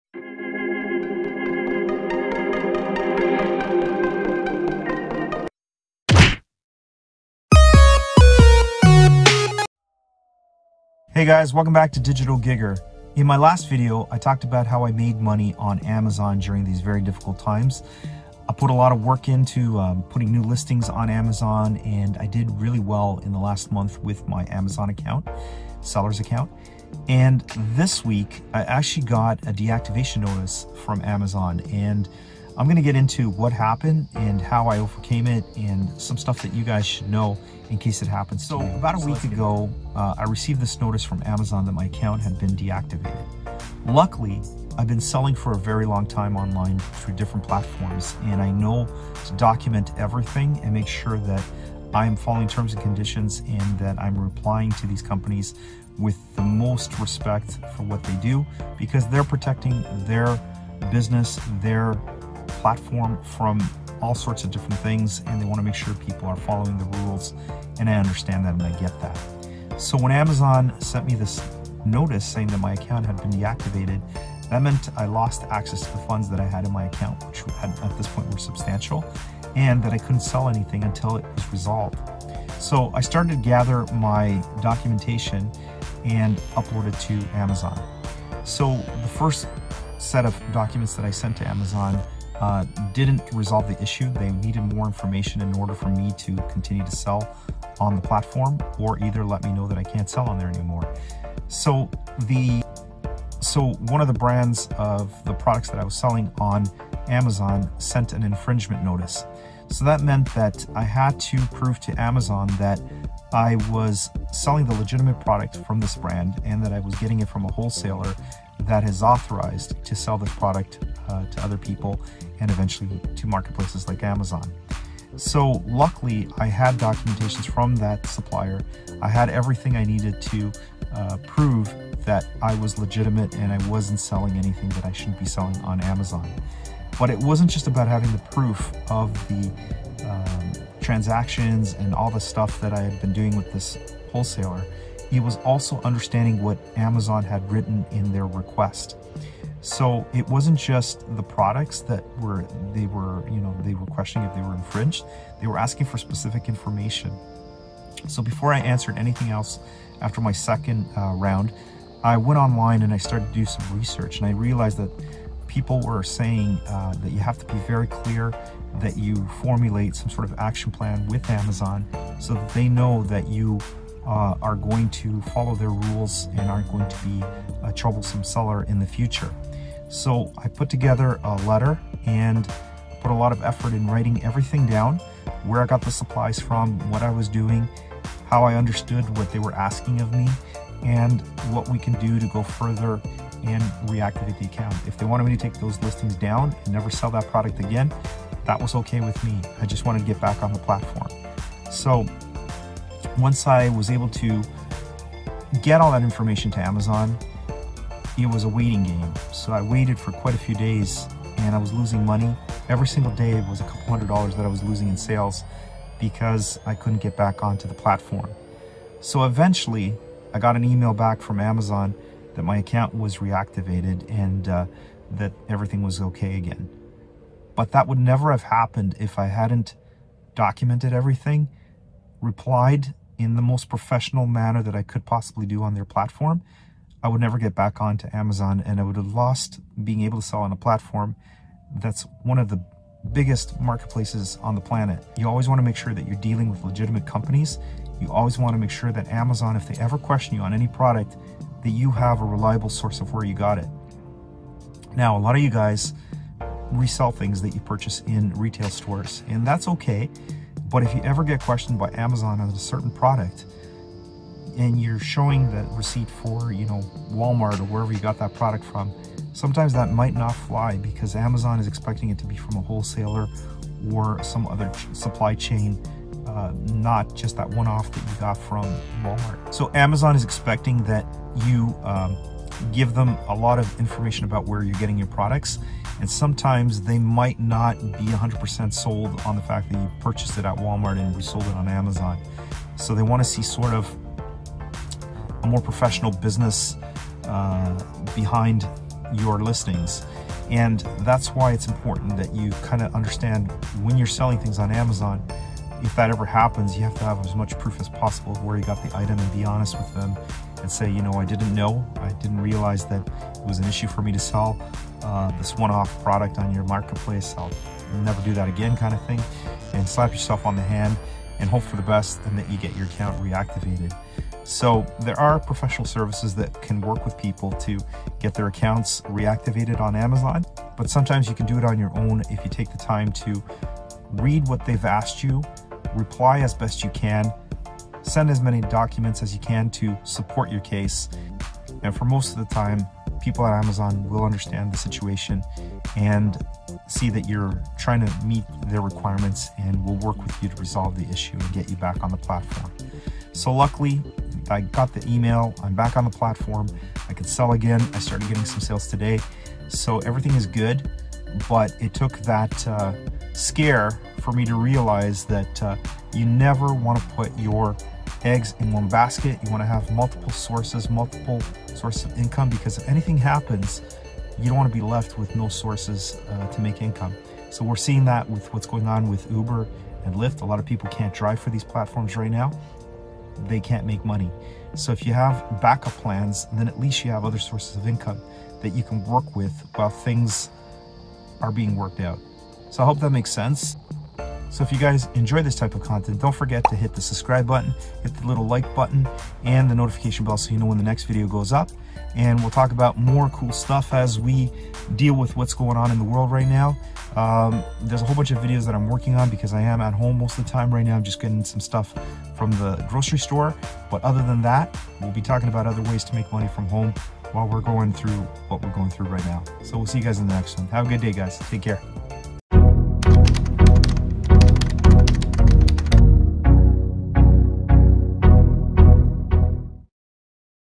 How to reactivate suspended Amazon Seller Account // And get back to selling on Amazon
How to reactivate suspended Amazon Seller Account. It happened to me and this is how I got my account reactivated. Follow the steps in the video to get your Amazon Seller account reactivated and avoid the issues I faced.

If you want more detailed information on how to sell on Amazon and make money I suggest this book:
How to Sell on Amazon in 2020: 7 FBA Secrets That Turn Beginners into Best Sellers